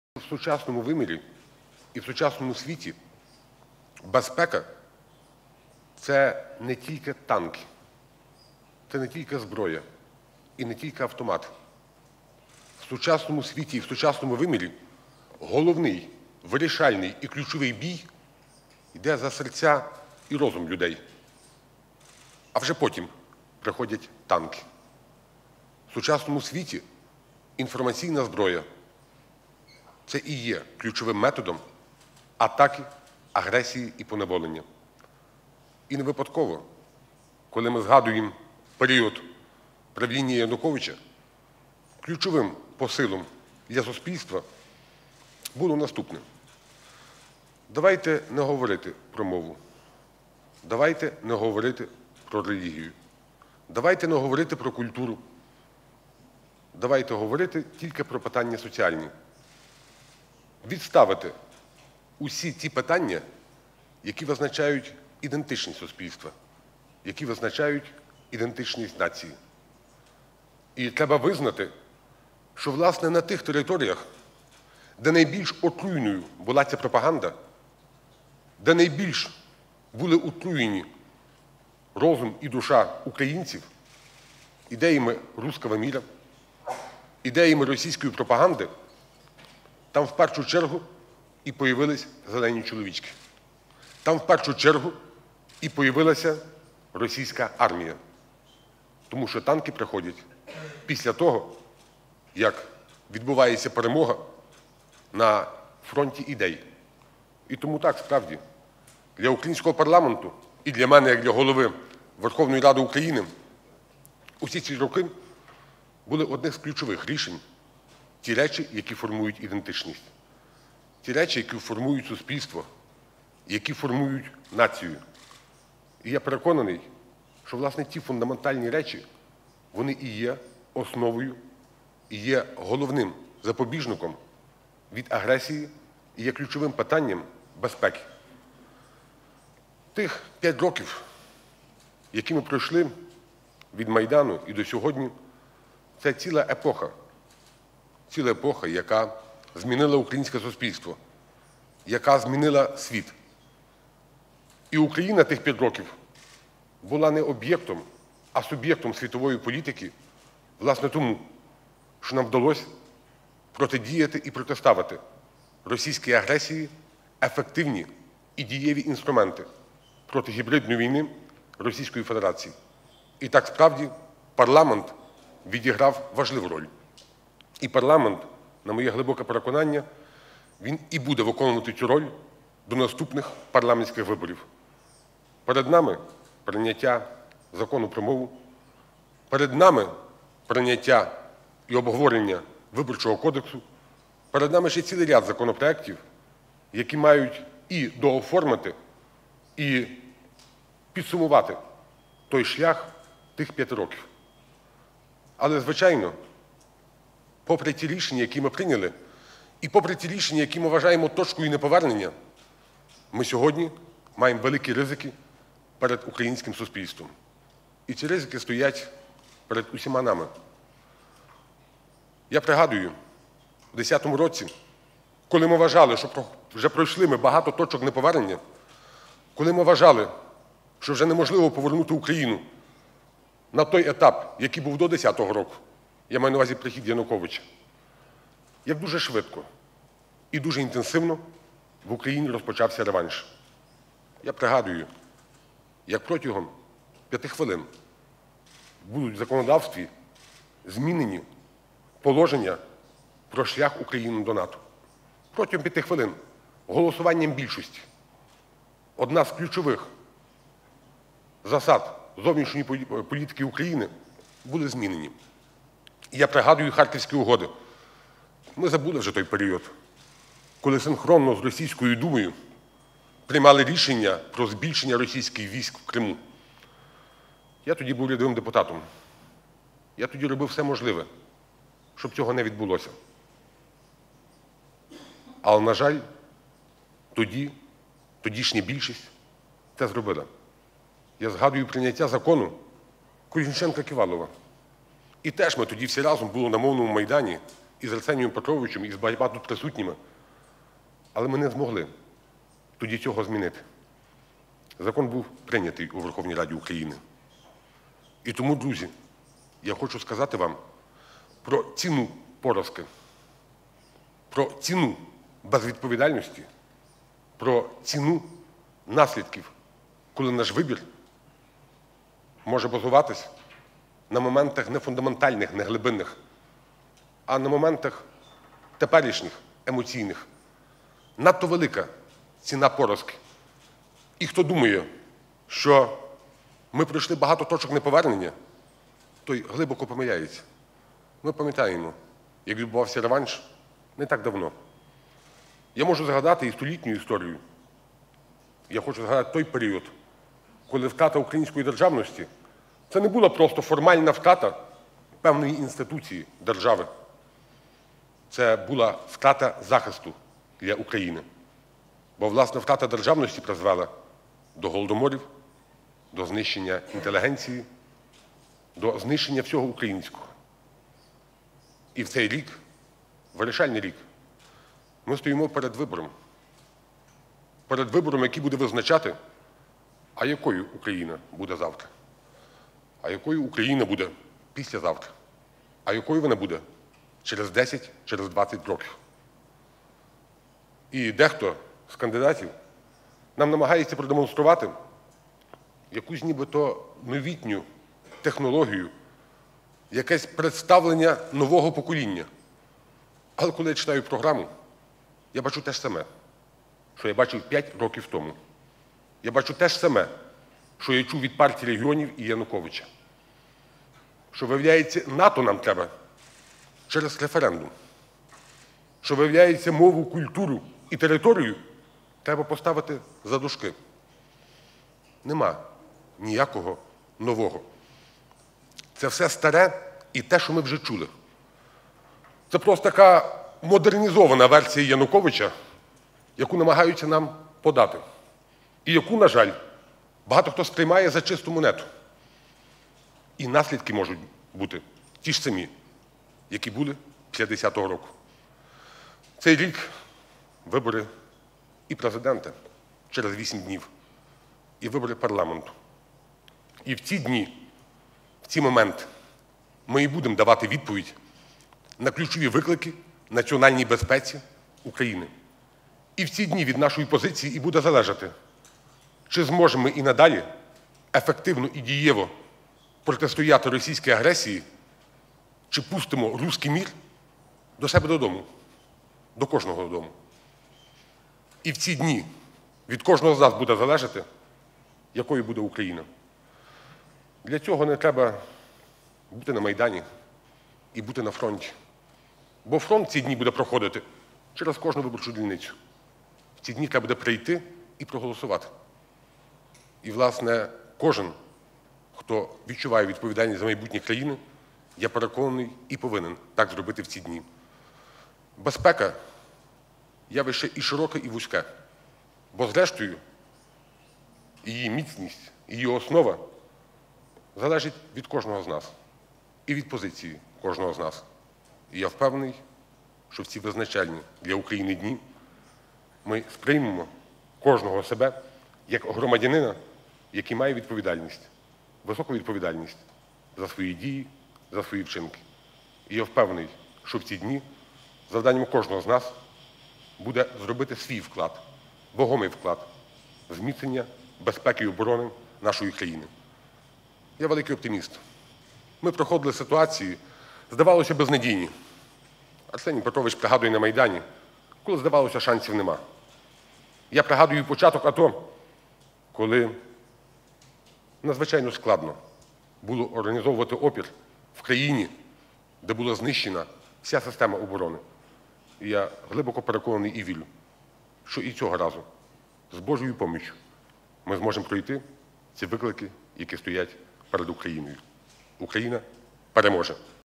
Фундаментальні речі, які є запобіжниками агресії і ключовими питаннями безпеки
Все, що формує ідентичність, суспільство і націю, - фундаментальні речі, які є запобіжниками агресії і ключовими питаннями безпеки.

На цьому наголосив Голова Верховної Ради України, представник "Народного фронту" Андрій Парубій, який взяв участь у Київському Безпековому Форумі "Невтомна хвиля. Стратегічний вибір України та Заходу" у п’ятницю, 12 квітня.

Під час свого виступу він підкреслив, що "незворотність зовнішньополітичного курсу України – це питання не лише безпеки і майбутнього України, але рівно ж це і питання безпеки Європейського Союзу, і всього вільного світу".

За словами Андрія Парубія, "у сучасному світі і сучасному вимірі безпека – це не лише танки чи зброя – головний і вирішальний бій йде за серця і розум людей, а вже потім приходять танки".

"У сучасному світі інформаційна зброя – це ключовий метод атаки, агресії і поневолення", - підкреслив він.

Голова парламенту нагадав, що за правління Януковича ключовим посилом для суспільства було не говорити про мову, про культуру, а лише про соціальні питання - відставити усі питання, які визначають ідентичність суспільства, визначають ідентичність нації.

У цьому контексті він зазначив, що "на тих територіях, де найбільш отруйною була подібна пропаганда, де були отруєні розум і душа українців ідеями "руського міра" і російської пропаганди, там у першу чергу і появилися "зелені чоловічки" і російська армія".

"Тому що танки приходять після того, як відбувається перемога на фронті ідей, - наголосив він. - Для українського парламенту і для мене як для Голови Верховної Ради України усі ці роки були одними з ключових ті речі, які формують ідентичність, які формують суспільство, націю. Власне, ці фундаментальні речі є основою і запобіжником агресії і є ключовими питаннями безпеки".

Андрій Парубій підкреслив, що ті п’ять років, що пройшли від Майдану,  - "це ціла епоха, яка змінила суспільство, яка змінила світ".

"Україна була не об’єктом, а суб’єктом світової політики, тому що змогла протидіяти російській агресії, протиставити ефективні і дієві інструменти проти гібридної війни РФ", - зазначив він.

На переконання Голови Верховної Ради, парламент відіграв у цьому надзвичайно важливу роль і буде виконувати її до наступних парламентських виборів.

"Перед нами прийняття і закону про мову, прийняття і обговорення виборчого кодексу, необхідно оформити і підсумувати результати п’яти років", - уточнив він, зауваживши, що попри все, "маємо великі ризики перед українським суспільством".

Андрій Парубій пригадав, як у 2010 році дуже швидко і інтенсивно у країні розпочався реванш, коли всі вважали, що пройшли багато точок неповернення, - зокрема, "харківські угоди", коли синхронно з Російською Думою було проголосовано про збільшення російських військ у Криму.

"Я був тоді народним депутатом і робив усе, щоб цього це не відбулося. На жаль, тодішня більшість це зробила", - сказав він і нагадав також про невдалу протидію "Закону Колісниченка-Ківалова", коли також не змогли нічого зробити і закон був прийнятий.

"Я нагадав про ціну поразки, про ціну безвідповідальності, про ціну  наслідків, коли наш вибір може базуватися не на фундаментальних, не глибинних, а на моментах теперішніх, емоційних. Надто велика ціна поразки. І хто думає, що ми пройшли багато точок неповернення, той глибоко помиляється", - підкреслив Голова парламенту.

Голова Верховної Ради, досліджуючи столітню історію України, вказав на той період, коли  втрата державності була не тільки втратою інституції країни – це була втрата захисту для України.

"Втрата державності привела до Голодомору, до знищення інтелігенції, до знищення усього українського, - зауважив він. - Тому сьогодні, у цей вирішальний рік ми визначаємо, якою Україна буде завтра, післязавтра, через десять чи двадцять років".

Андрій Парубій охарактеризував програмні моменти одного з кандидатів як ті, що "були продемонстровані партією Януковича: в НАТО через референдум, мову і культуру поставити за дужки, - немає нового - це старе, що ми вже чули, модернізована версія Януковича, яку намагаються нам подати". За його словами, наслідки можуть бути ті ж самі, що і сто років тому.

Тому, переконаний Голова парламенту, "важливо усвідомити, чи будемо давати відповідь на ключові виклики національній безпеці України, чи зможемо протистояти, чи пустимо руський мір до себе додому, до кожного".

"У ці дні від кожного з нас залежатиме, якою буде Україна. Для цього не треба бути на Майдані чи фронті, бо фронт буде проходити на кожній дільниці", - наголосив він і закликав кожного, хто відчуває відповідальність за долю України, прийти і проголосувати.

Приєднуйся!